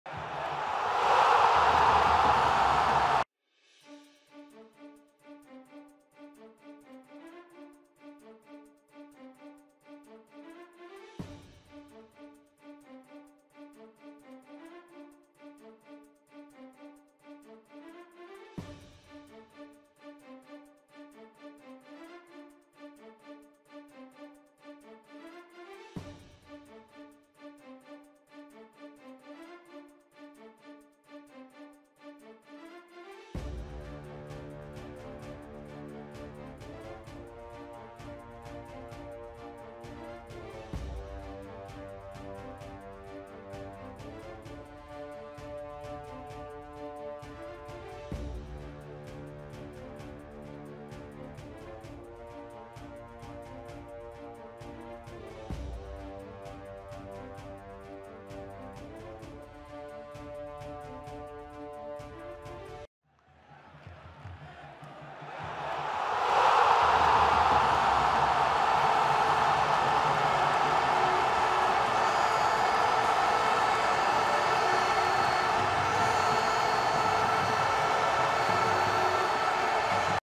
Polska - Senegal 1:2 Ciekawostki po meczu
Niestety przegrywamy w naszym pierwszym meczu na mundialu w Rosji. Wynik 1:2 smuci, ale nie mogłem nie przygotować filmu z pomeczowymi ciekawostkami.

PAMIĘTAJCIE, ŻE TO NIE SKRÓT TYLKO CIEKAWOSTKI

Fajne? Daj suba i dzwona i wbij na Insta